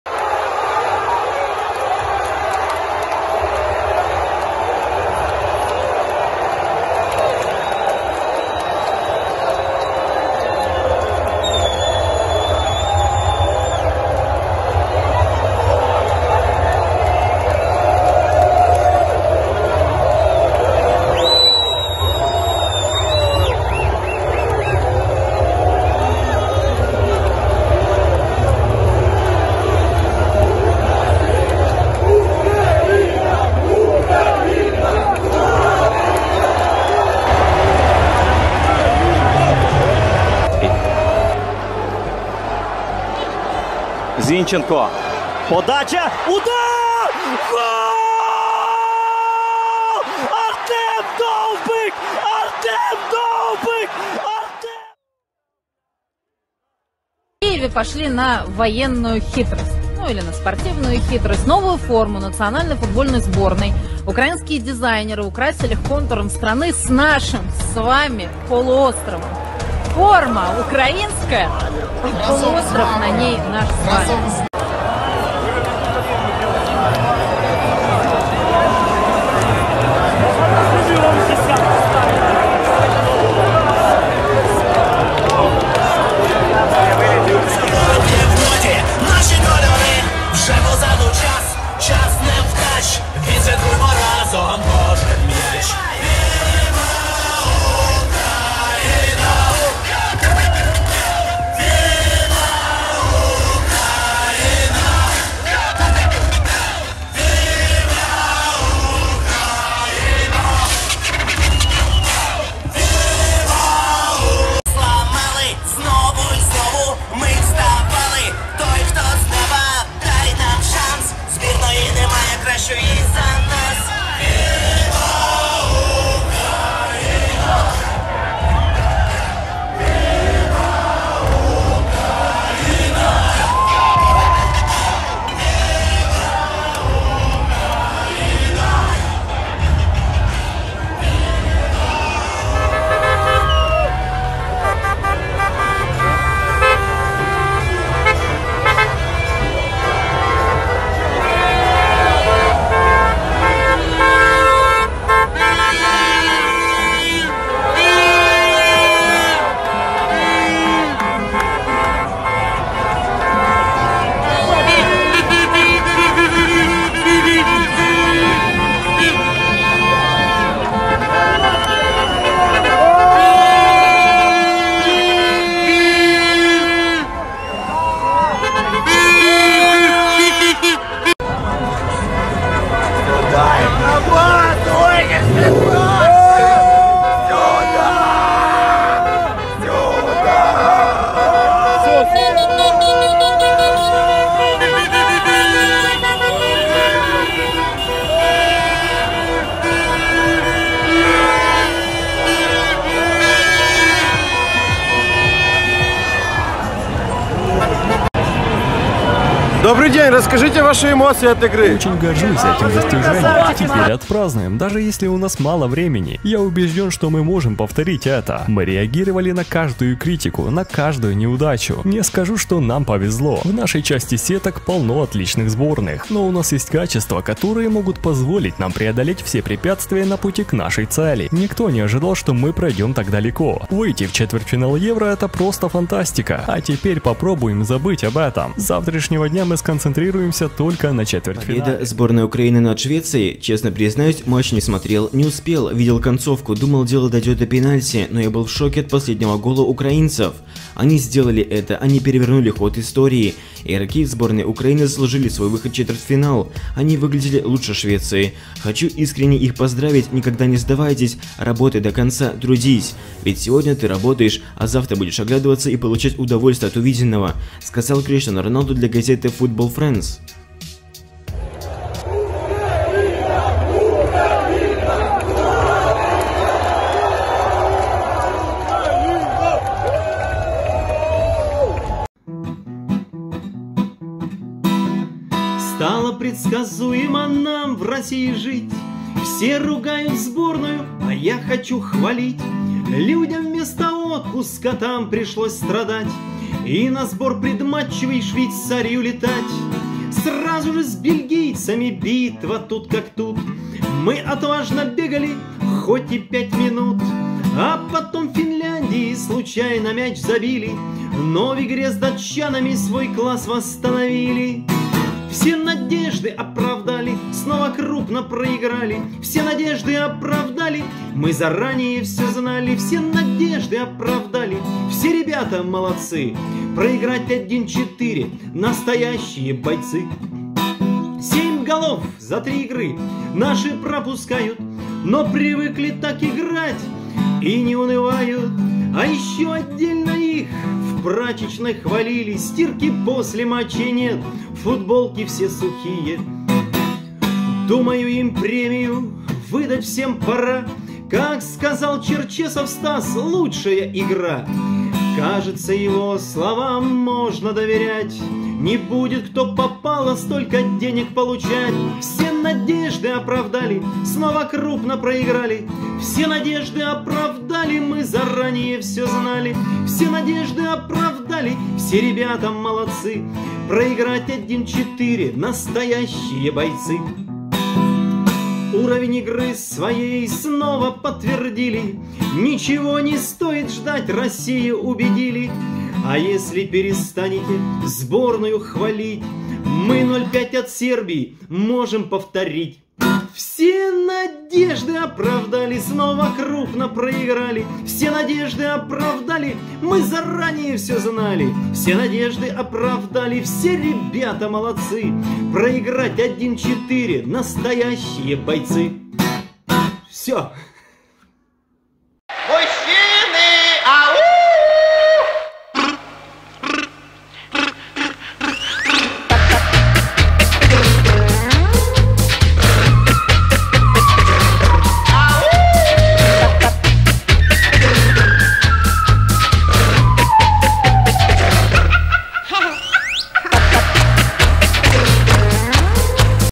Поздравляем Украину с Победой!!!
Победа сборной Украины над Швецией и впервые Украина в 1/4 финала чемпионата Европы